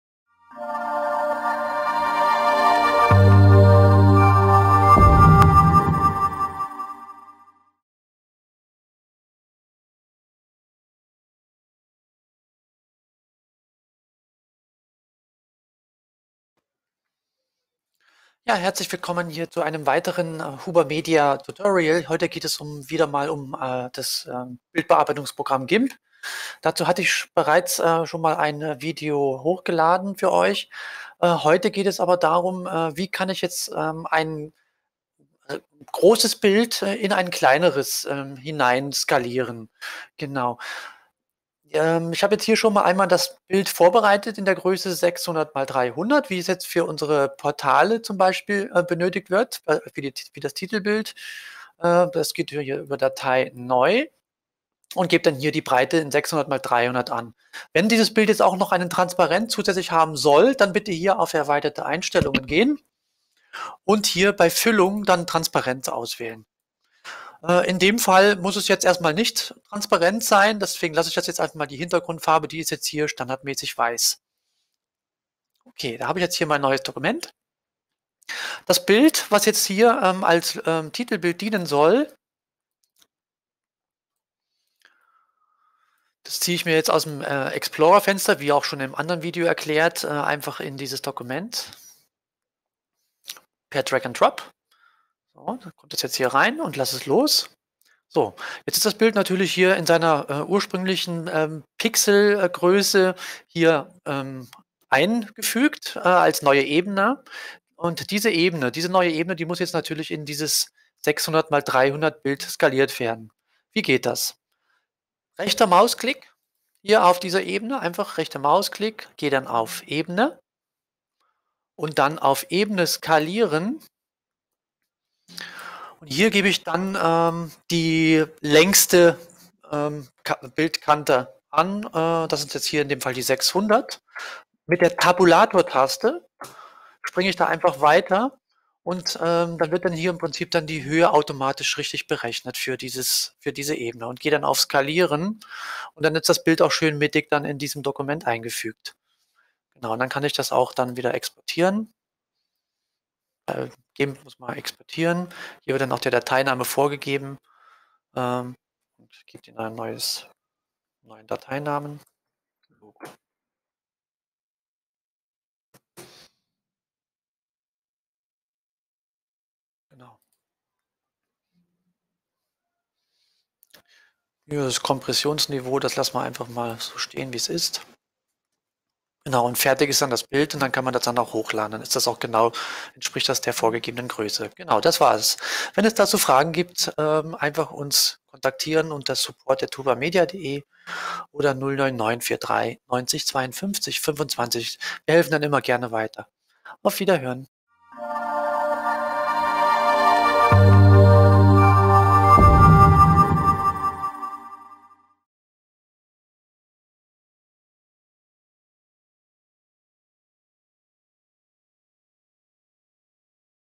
Gimp: Wie skaliere ich ein großes Bild in ein kleineres Bild Dokument?
Wie kann ich ein größeres Bild in ein ein kleineres vorgegebenes Bilddokument einfügen, ohne Verlust der ursprünglichen Proportionen.

Dafür ist Gimp, dass auch mit Ebenen umgehen kann, bestens geeignet.